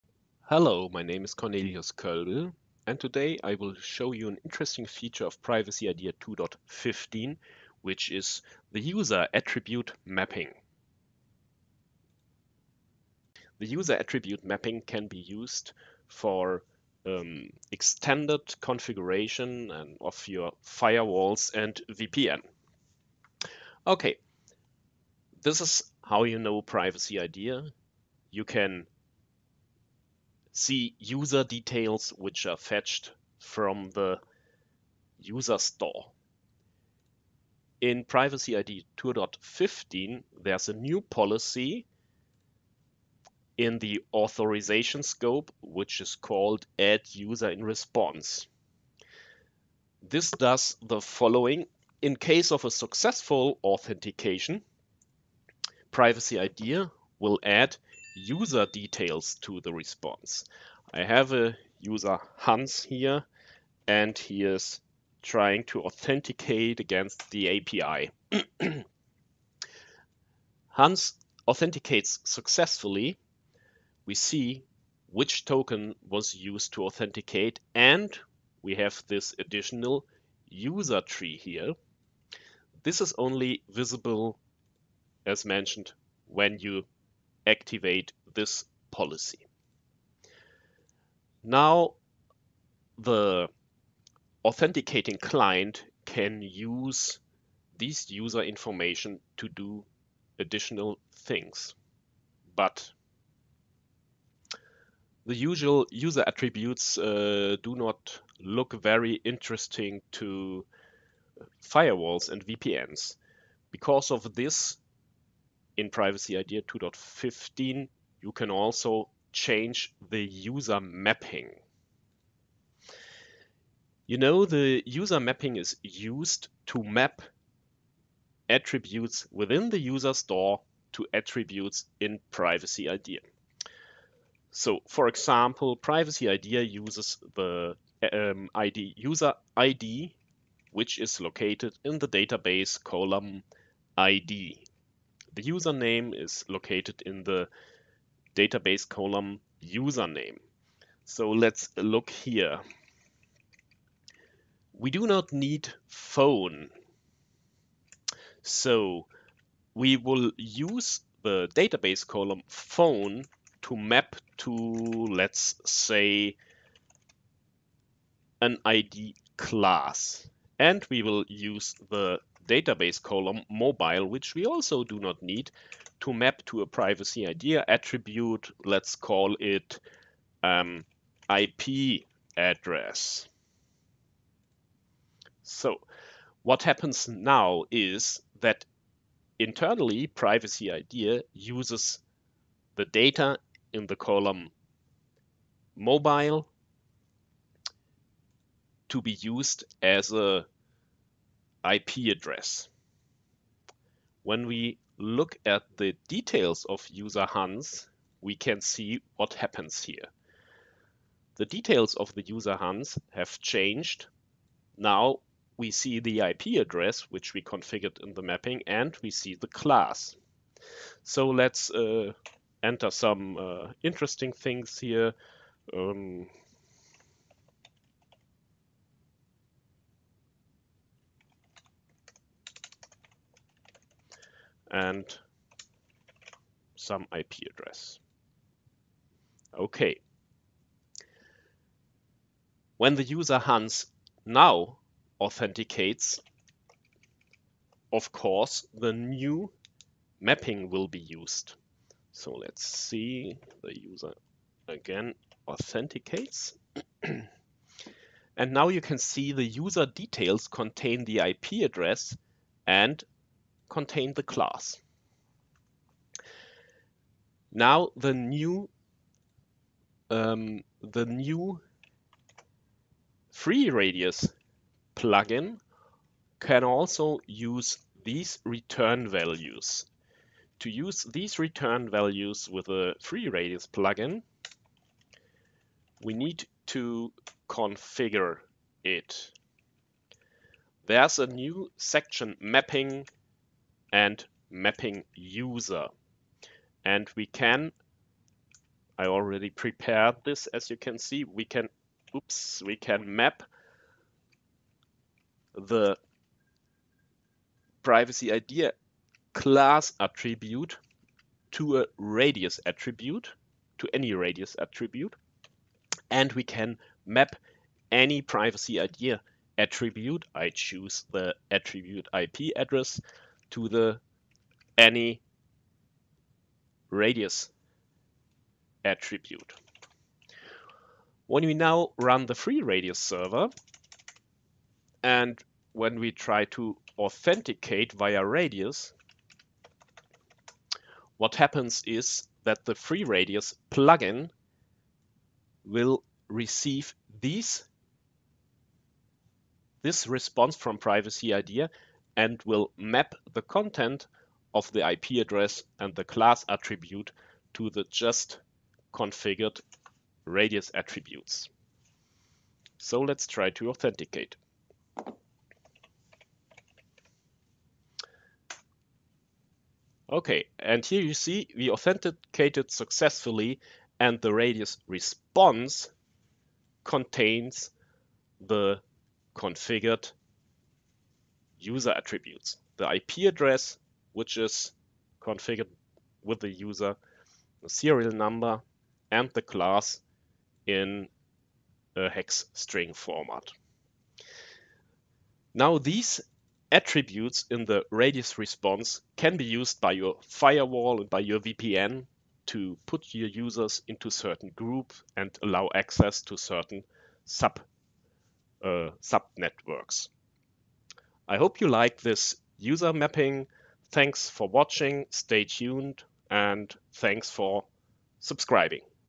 privacyIDEA User Attribute Mapping
When doing 2FA at Firewalls and VPNs providing additional attributes of the users is crucial to putting the user in groups or allowing access to certain sub networks.

Thus privacyIDEA 2.15 provides a sophisticated user attribute mapping, which is passed via the RADIUS protocol to the firewall.